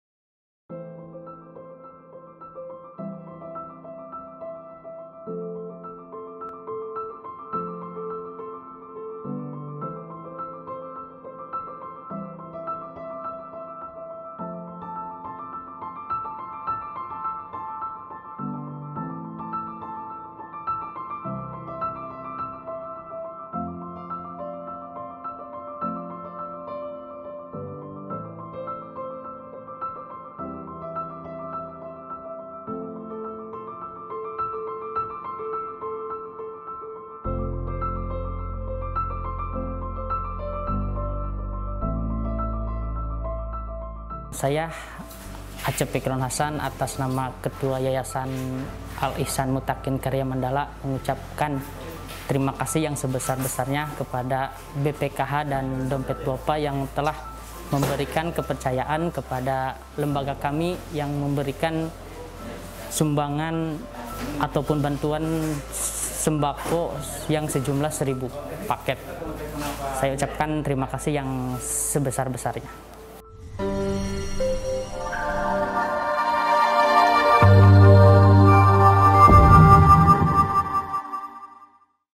Dompet Dhuafa Bersama BPHK mendistribusikan 3000 Paket Sembako
3000 Paket Sembako Keluaga donasi dari BPKH telah di distribusikan oleh DMC Dompet Dhuafa untuk wilayah Tasikmalaya.